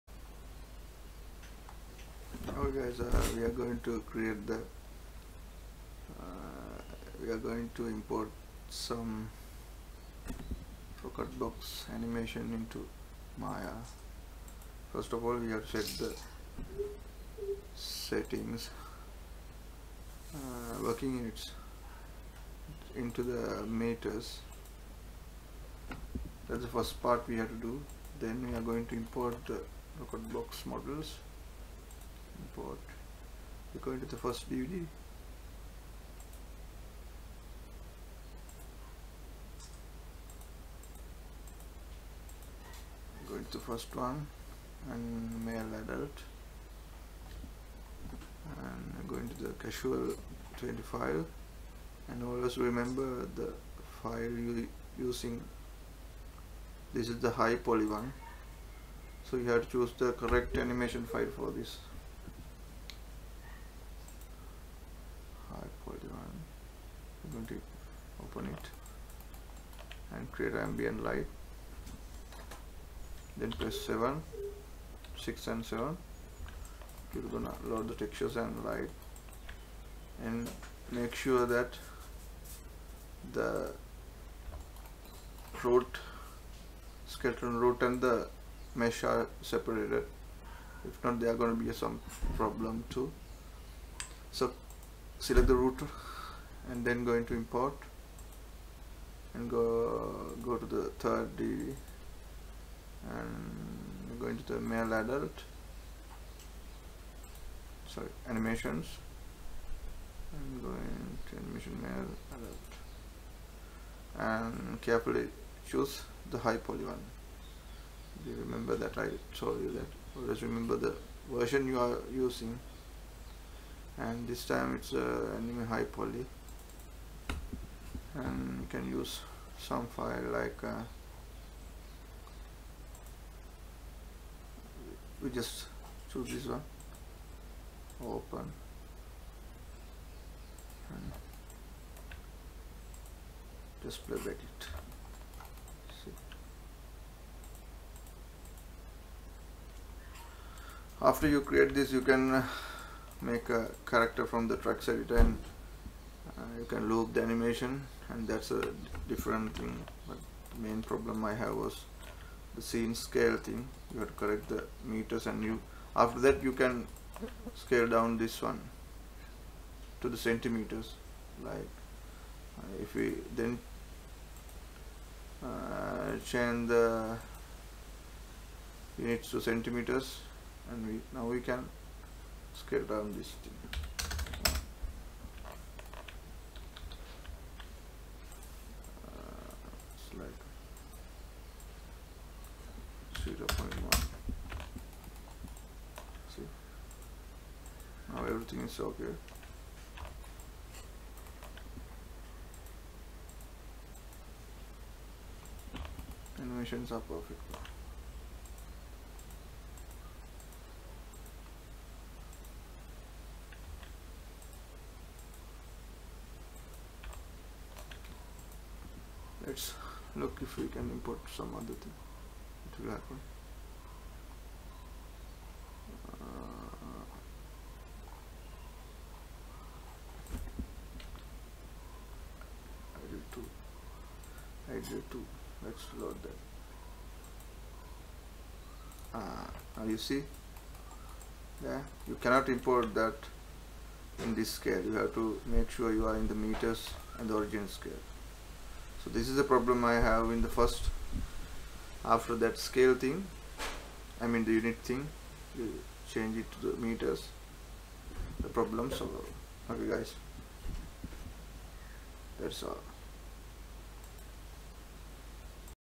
Rocket box animations in to maya (English)
importing Rocketbox models and animations in to Autodesk Maya correctly.(English)